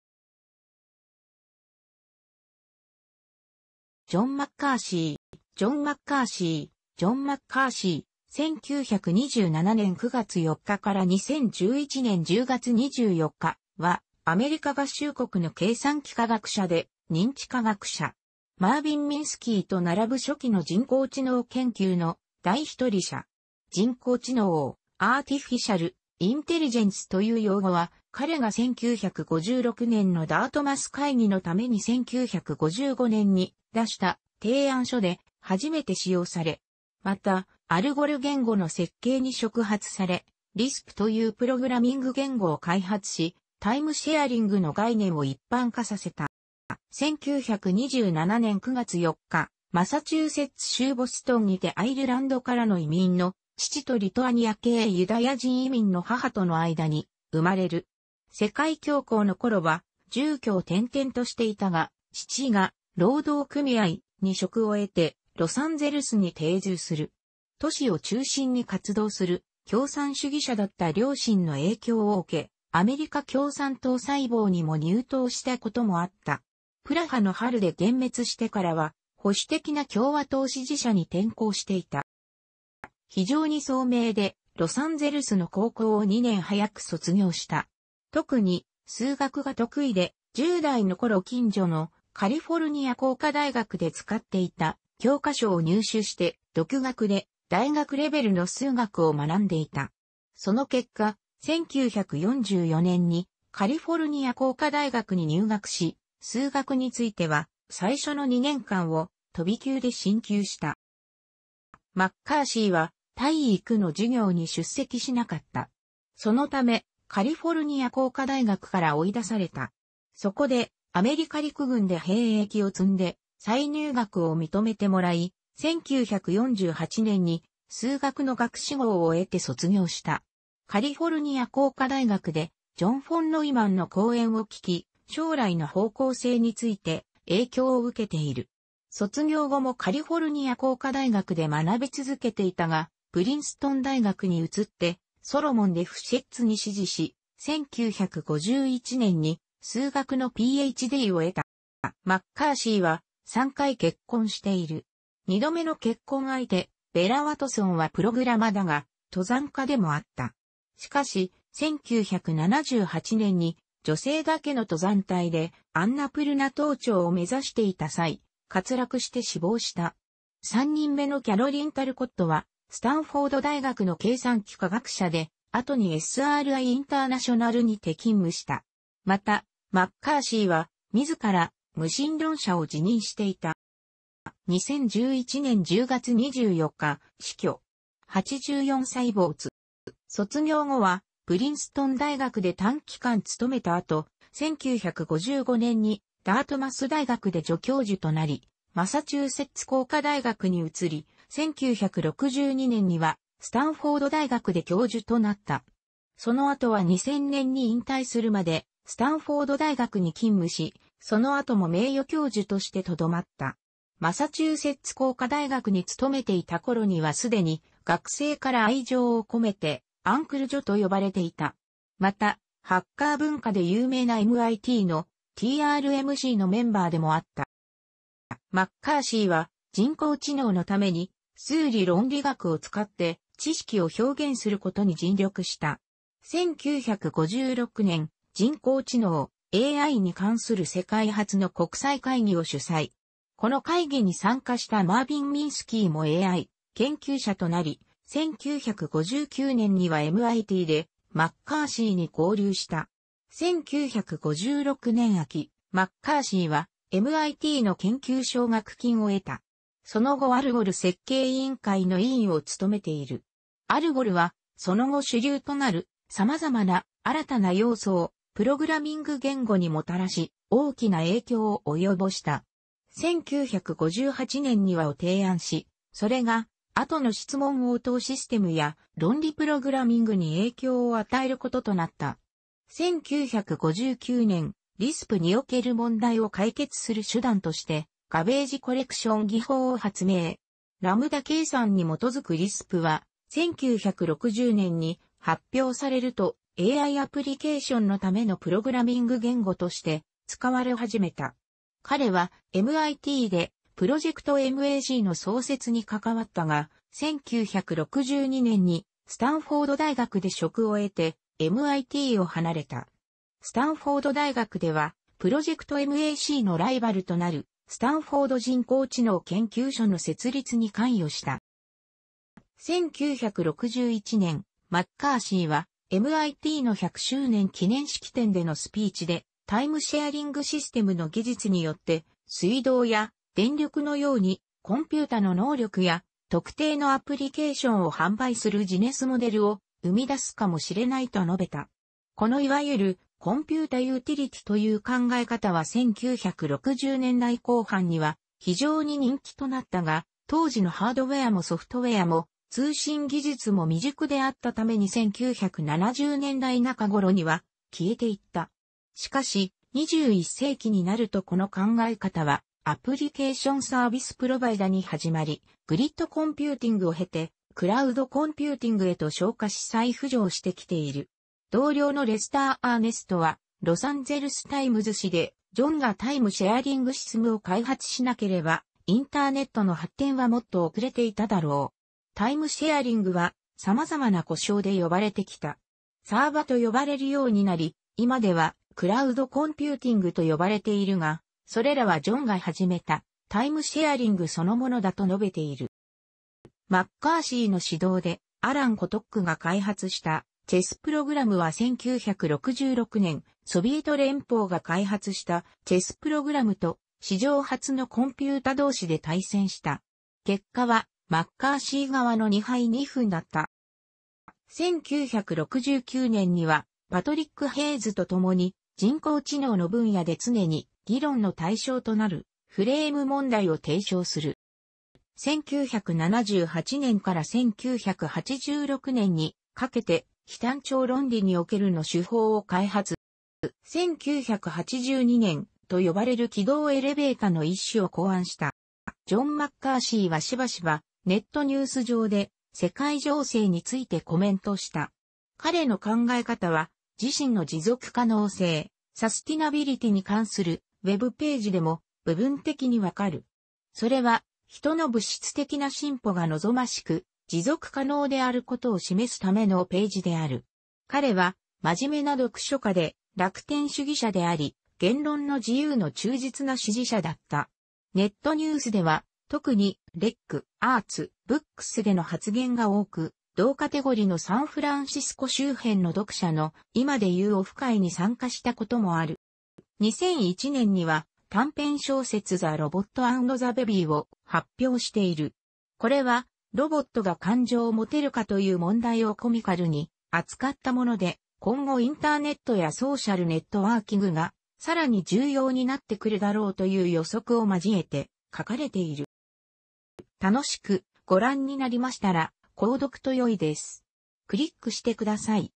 ジョン・マッカーシー
ジョン・マッカーシー（John McCarthy, 1927年9月4日 - 2011年10月24日）は、アメリカ合衆国の計算機科学者で認知科学者。マービン・ミンスキーとならぶ初期の人工知能研究の第一人者。「人工知能; Artificial Intelligence」という用語は彼が1956年のダートマス会議のために1955年に出した提案書で初めて使用された。また、ALGOL言語の設計に触発され、LISPというプログラミング言語を開発し、タイムシェアリングの概念を一般化させた。

1927年9月4日、マサチューセッツ州ボストンにてアイルランドからの移民の父とリトアニア系ユダヤ人移民の母との間に生まれる。世界恐慌のころは住居を転々としていたが、父が労働組合 () に職を得て、ロサンゼルスに定住する。都市を中心に活動する共産主義者だった両親の影響を受け、アメリカ共産党細胞にも入党したこともあった。プラハの春で幻滅してからは保守的な共和党支持者に転向していた。

非常に聡明で、ロサンゼルスの高校を2年早く卒業した。特に、数学が得意で、十代のころ近所のカリフォルニア工科大学で使っていた教科書を入手して、独学で大学レベルの数学を学んでいた。その結果、1944年にカリフォルニア工科大学に入学し、数学については最初の2年間を飛び級で進級した。

マッカーシーは、体育の授業に出席しなかった。そのため、カリフォルニア工科大学から追い出された。そこで、アメリカ陸軍で兵役を積んで、再入学を認めてもらい、1948年に数学の学士号を得て卒業した。カリフォルニア工科大学でジョン・フォン・ノイマンの講演を聴き、将来の方向性について影響を受けている。卒業後もカリフォルニア工科大学で学び続けていたが、プリンストン大学に移ってソロモン・レフシェッツに師事し、1951年に数学のPh.D.を得た。

マッカーシーは、3回結婚している。2度目の結婚相手ベラ・ワトソンはプログラマだが、登山家でもあった。しかし、1978年に女性だけの登山隊でアンナプルナ登頂を目指していた際、滑落して死亡した。3人目のキャロリン・タルコットは、スタンフォード大学の計算機科学者で、後にSRIインターナショナルにて勤務した。また、マッカーシーは、自ら無神論者を自認していた。

2011年10月24日、死去。84歳没。

卒業後はプリンストン大学で短期間勤めた後、1955年にダートマス大学で助教授となり、マサチューセッツ工科大学に移り、1962年にはスタンフォード大学で教授となった。その後は2000年に引退するまでスタンフォード大学に勤務し、その後も名誉教授としてとどまった。マサチューセッツ工科大学に勤めていたころには既に学生から愛情をこめて「アンクル・ジョン」と呼ばれていた。また、ハッカー文化で有名なMITの (TRMC) のメンバーでもあった。

マッカーシーは人工知能のために数理論理学を使って知識を表現することに尽力した。1956年、人工知能 (AI) に関する世界初の国際会議を主催。この会議に参加したマービン・ミンスキーもAI研究者となり、1959年にはMITでマッカーシーに合流した。1956年秋、マッカーシーはMITの研究奨学金を得た。その後ALGOL設計委員会の委員を務めている。ALGOLはその後主流となる様々な新たな要素をプログラミング言語にもたらし、大きな影響を及ぼした。1958年には を提案し、それが後の質問応答システムや論理プログラミングに影響を与えることとなった。1959年、LISPにおける問題を解決する手段として「ガベージコレクション」技法を発明。ラムダ計算に基づくLISPは、1960年に発表されると、AIアプリケーションのためのプログラミング言語として使われはじめた。彼は MIT で Project MAC の創設に関わったが、1962年にスタンフォード大学で職を得て MIT を離れた。スタンフォード大学では Project MAC のライバルとなるスタンフォード人工知能研究所の設立に関与した。

1961年、マッカーシーは MIT の100周年記念式典でのスピーチで、タイムシェアリングシステムの技術によって（水道や電力のように）コンピュータの能力や特定のアプリケーションを販売するビジネスモデルを生み出すかもしれないと述べた。このいわゆる「コンピュータユーティリティ」という考え方は1960年代後半には非常に人気となったが、当時のハードウェアもソフトウェアも通信技術も未熟であったために19...